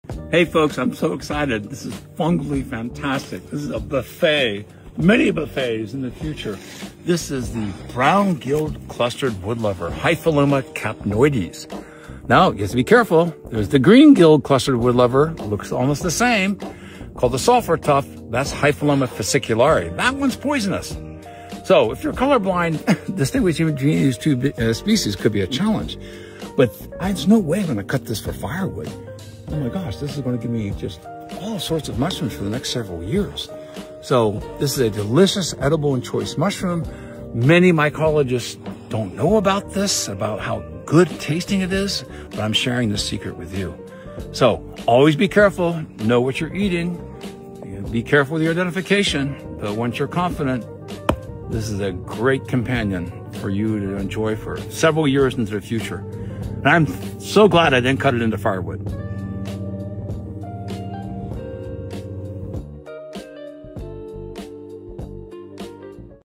Hypholoma capnoides
The smoky brown clustered wood lover, Hypholoma capnoides, is a delicious mushroom that few know about. This species has purplish brown spores. Be careful, there are some poisonous lookalikes — like the green filled clustered wood lover, Hypholoma fasciculare, aka the sulphur tuft. Even more dangerous and toxic is a rusty brown spored orangish mushroom species in the Galerina marginata group and can be DEADLY!

Take a spore print! And be absolutely accurate in your identification before consuming.

This mushroom is for experts! Be CAREFUL!

See my latest book for more information.

Mycology is edgy….and so much fun!

Filming and photos by Pamela Kryskow, MD